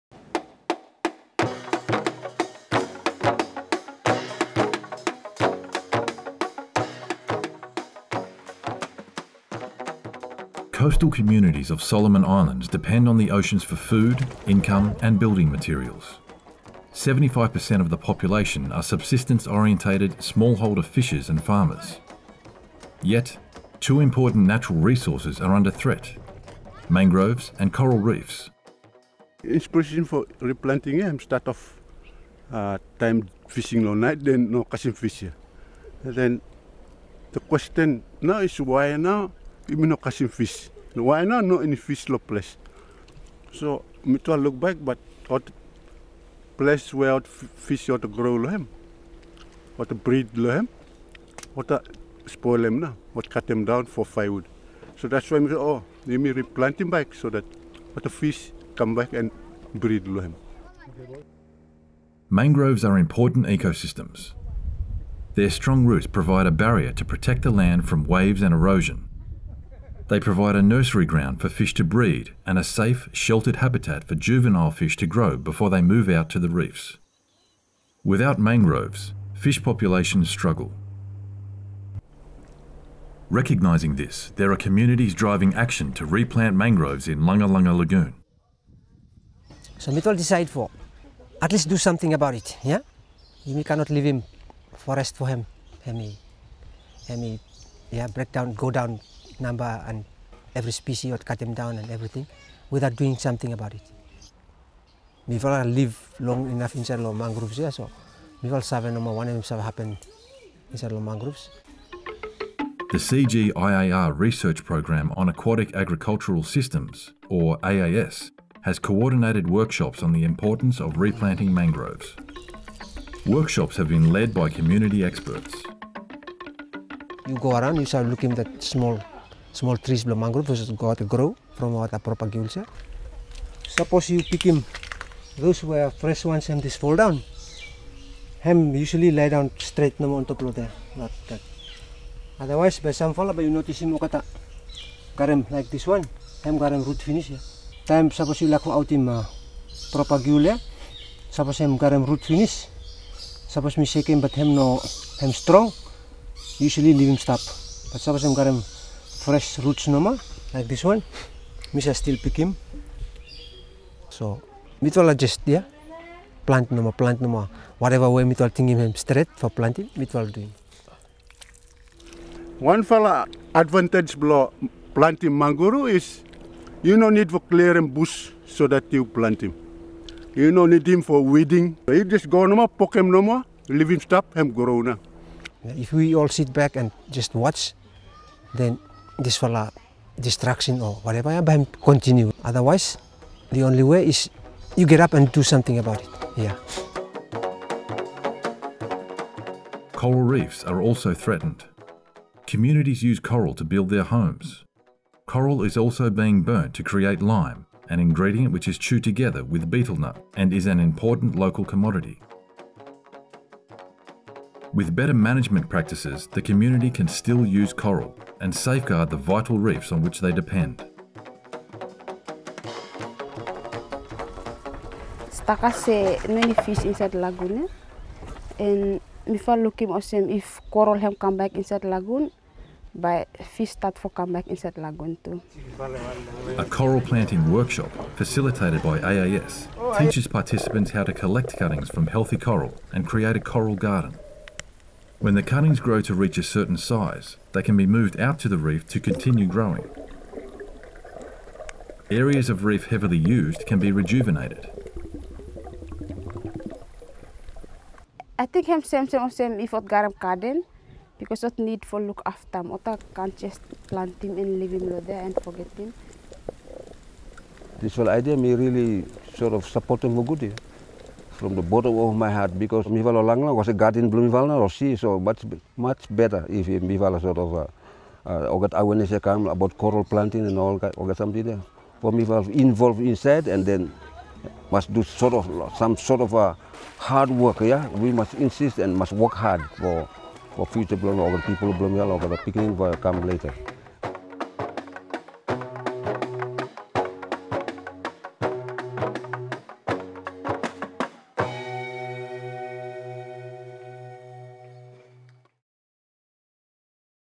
Replanting Mangroves and coral in Solomon Islands
Coastal communities in Malaita, Solomon Islands are leading training workshops to teach participants how to replant mangroves and corals. These critical ecosystems provide breeding grounds and habitats for marine fish and other aquatic animals that support the livelihoods of thousands of subsistence fishers and their families.

However, they are also a source of building materials, firewood and lime, and are under threat from overharvesting. Replanting mangrove forests and replenishing coral reefs will help to secure these natural resources and their vast benefits in to the future.